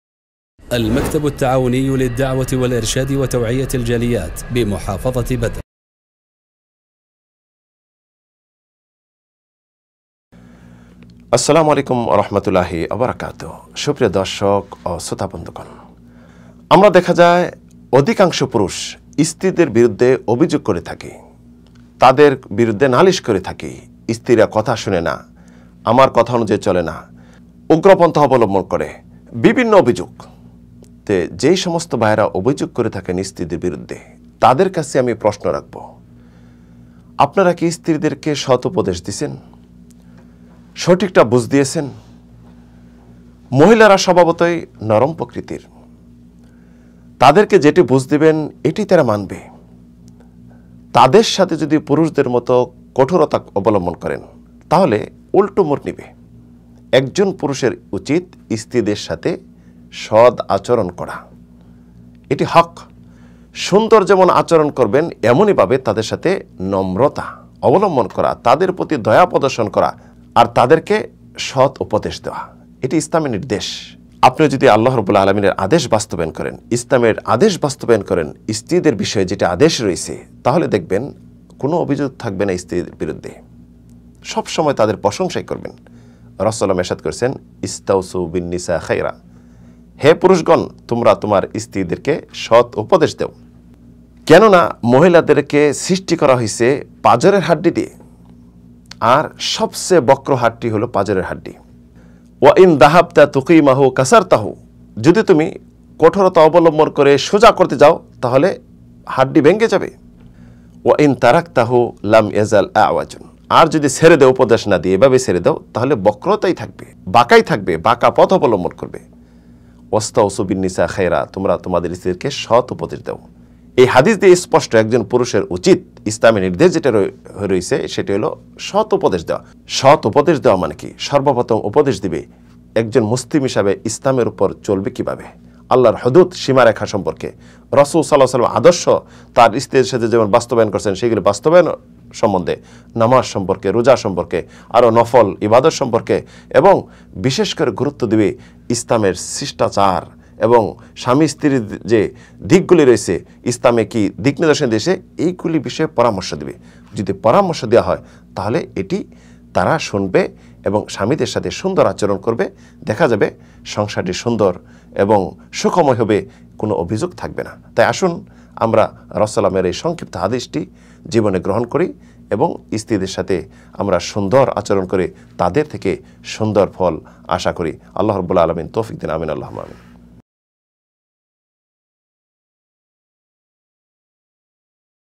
অভিযোগ মুক্ত স্ত্রী গঠনে স্বামীর করণীয়! BANGLA WAZ LECTURE by Rafiq Bin Lokman Madani
অভিযোগ মুক্ত স্ত্রী গঠনে স্বামীর করণীয়!
দাম্পত্য জীবনে সুখী হতে প্রিয় নবীর স্ত্রীদের সাথে ঘটে যাওয়া এই গল্প থেকে শিক্ষা গ্রহণ করুন
অধিকাংশ জাহান্নামের অধিবাসী মহিলারা কেন? (Jahannami Mohila)